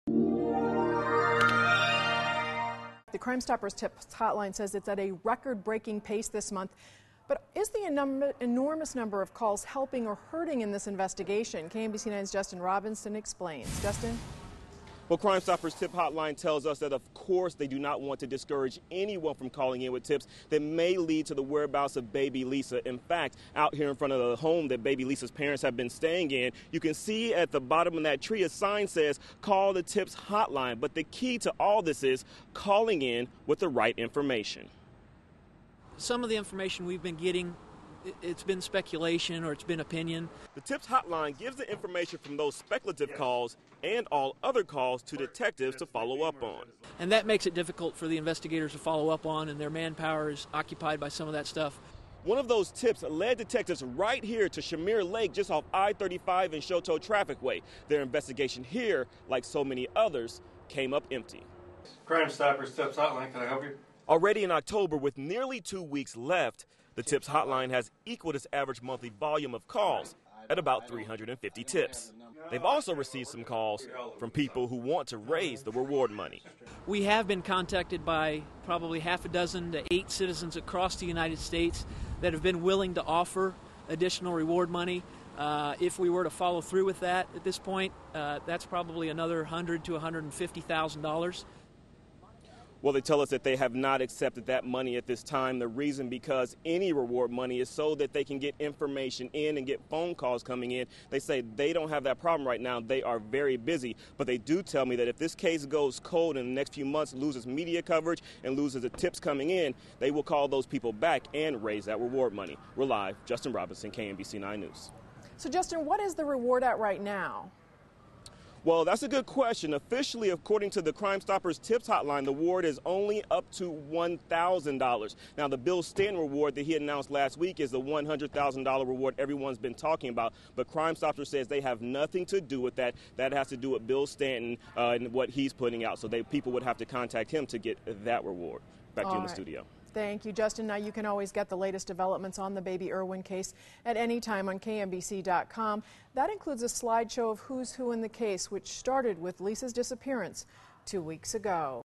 Baby Lisa Calls Flood TIPS Hotline
KMBC 9's Justin Robinson said the Crime Stoppers TIPS Hotline has been getting hundreds of calls about Lisa Irwin, but some of those calls are little more than theories and speculation.